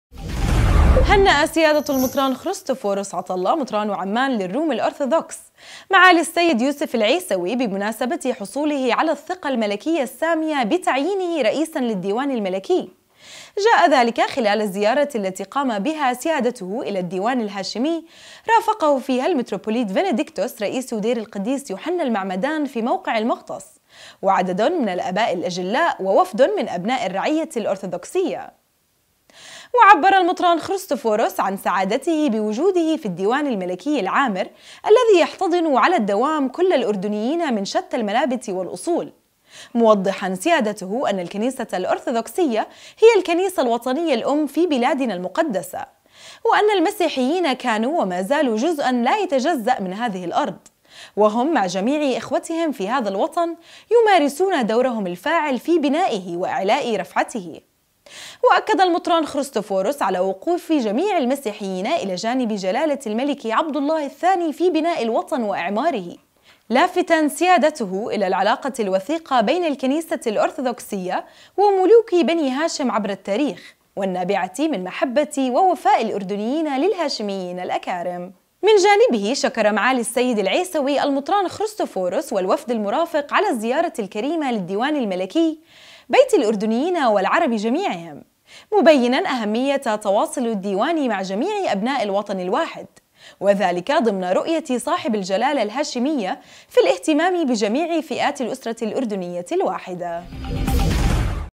نورسات الأردن : المطران خريستفورس والوفدُ المرافق يلتقي رئيسَ الديوانِ الملكي
هنأَ سيادةُ المطران خريستوفوروس عطالله مطرانُ عمان للرومِ الارثوذكس معالي السيد يوسف العيسوي بمناسبةِ حصولهِ على الثقةِ الملكيةِ السامية بتعيينهِ رئيساً للديوانِ الملكي ، جاء ذلك خلالَ الزيارةِ التي قامَ بها سيادتُهُ الى الديوانِ الهاشمي رافقَهُ فيها المتروبوليت فينيذكتوس رئيسُ ديرِ القديس يوحنا المعمدان في موقعِ المغطس وعددٌ من الآباءِ الأجلاءْ ووفدٌ من أبناءِ الرعيةِ الأرثوذكسية .. وعبّرَ المطران خريستوفوروس عن سعادتهِ بوجودهِ في الديوانِ الملكيِ العامر الذي يحتضنُ على الدوام كلَّ الأردنيينَ من شتى المنابتِ والأصول ، موضحاً سيادتُهُ أنَّ الكنيسةَ الأرثوذكسية هي الكنيسةُ الوطنيةُ الأمْ في بلادِنا المقدّسة ، وأنَّ المسيحيينَ كانوا وما زالوا جُزءاً لا يتجزأُ من هذهِ الأرض ، وهمْ مع جميعِ إخوتهمْ في هذا الوطن يمارسونَ دورَهم الفاعل في بنائهِ وإعلاءِ رفعته ، وأكدَّ المطران خريسفورس على وقوفِ جميعِ المسيحيينَ إلى جانبِ جلالةِ الملكِ عبدالله الثاني في بناءِ الوطنِ وإعمارِه ، لافتاً سيادتُهُ الى العلاقةِ الوثيقةِ بين الكنيسةِ الأرثوذكسية وملوكِ بني هاشم عبرَ التاريخ ، والنابعةِ من محبةِ ووفاءِ الأردنيينَ للهاشميينَ الأكارم  .. من جانبه شكر معالي السيد العيسوي المطران خريستوفورس والوفدَ المرافقْ على الزيارةِ الكريمةِ للديوانِ الملكي ، بيتِ الاردنيينَ والعربِ جميعِهم ، مبيناً أهميةَ تواصلِ الديوانِ مع جميعِ أبناءِ الوطنِ الواحد ، وذلكَ ضمنَ رؤيةِ صاحبِ الجلالةِ الهاشمية في الاهتمامِ بجميعِ فئاتِ الأسرةِ الأردنيةِ الواحده .